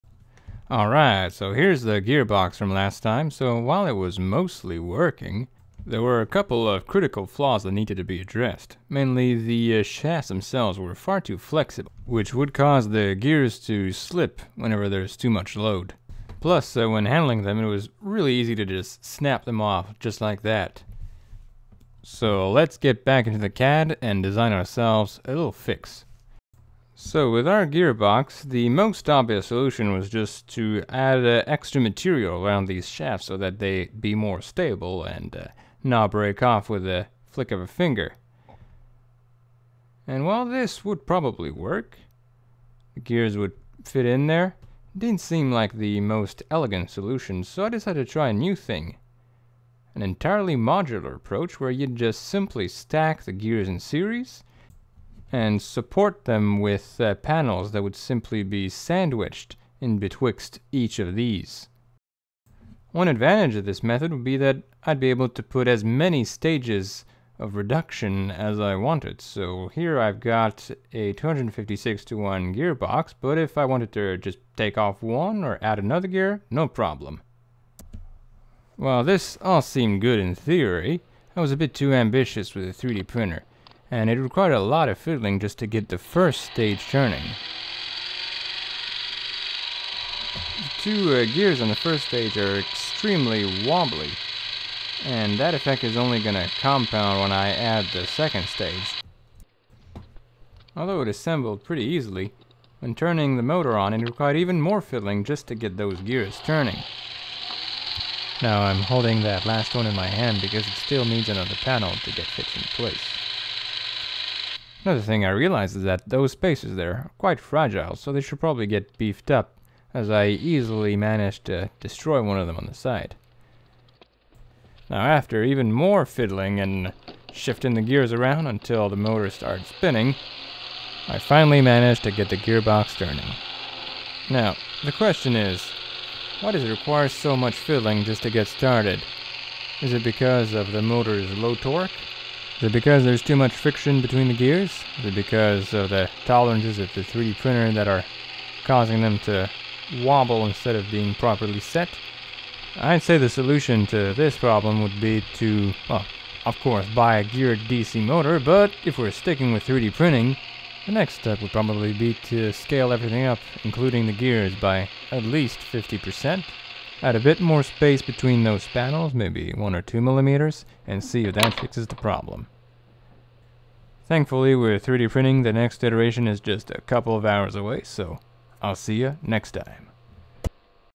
3D Printed Spur Gearbox #2: Modular Panels
Trying out a modular approach for the gearbox, with a set of panels allowing to easily reduce or augment the gear ratio.